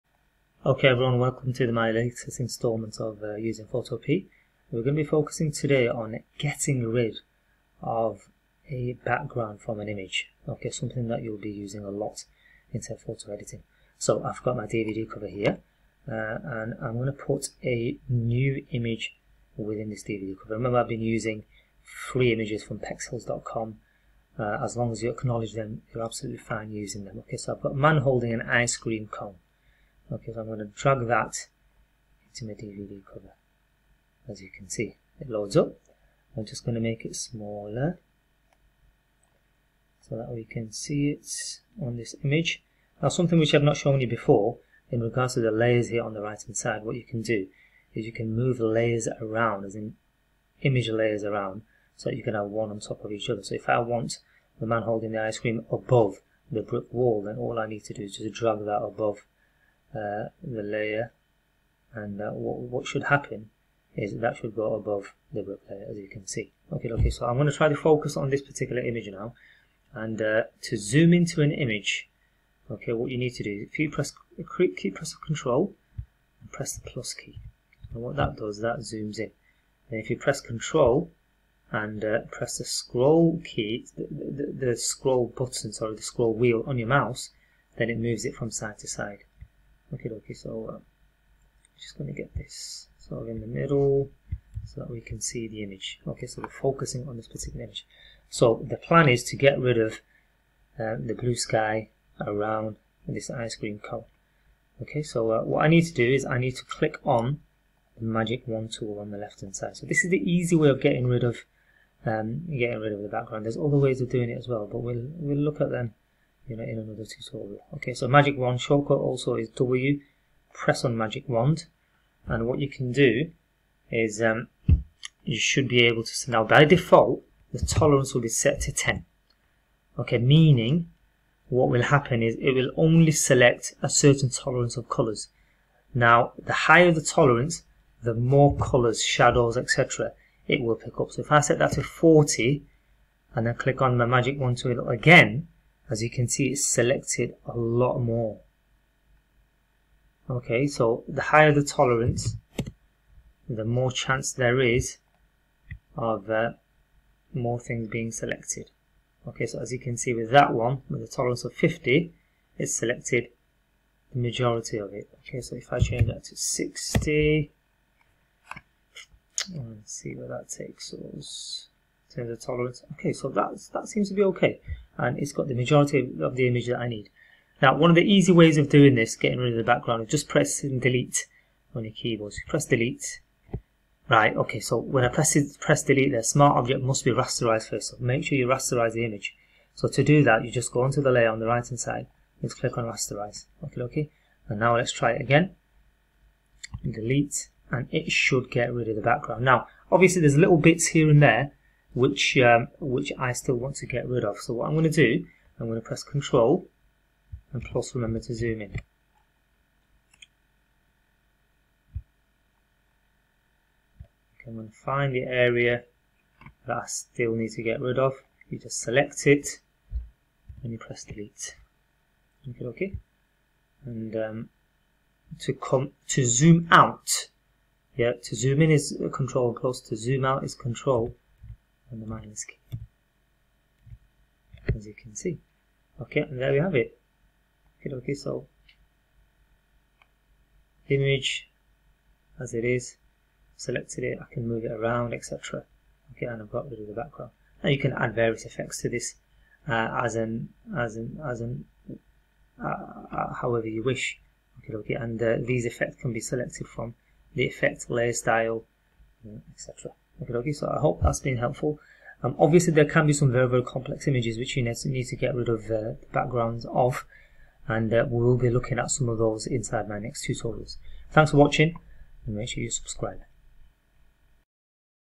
Photopea - Removing a Background from an Image Using the Magic Wand Tool
A quick short video tutorial on getting rid of a background from an image in Photopea!